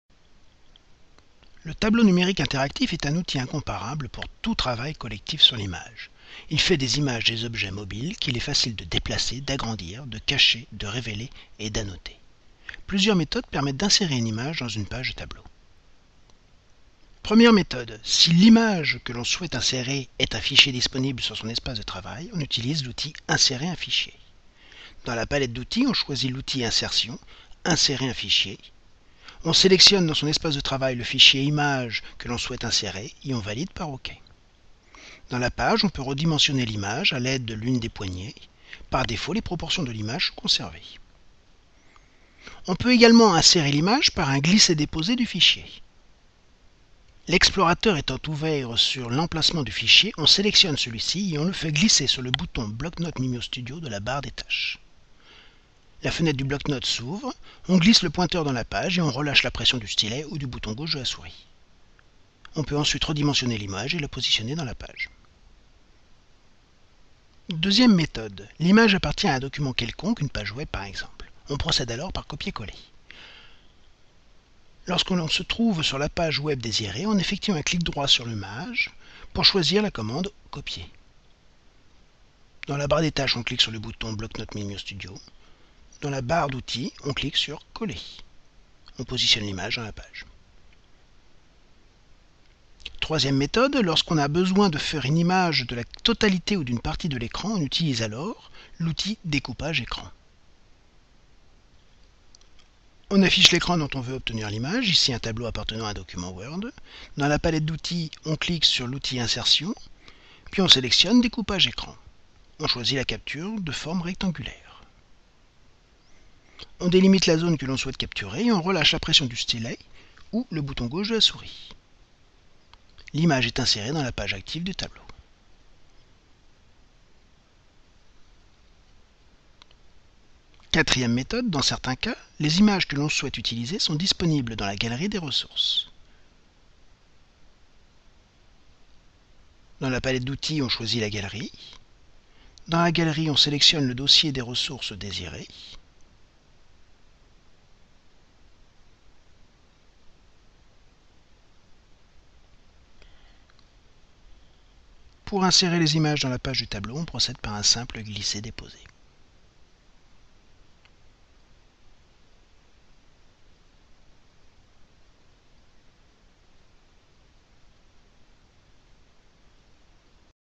Insérer des images dans une page du Bloc-notes MimioStudio
les différentes méthodes pour insérer une image dans une page de tableau (à l'aide de l'outil Insérer un fichier, par un Copier/Coller, à l'aide de l'outil de capture, à partir de la Galerie ou des pièces jointes. )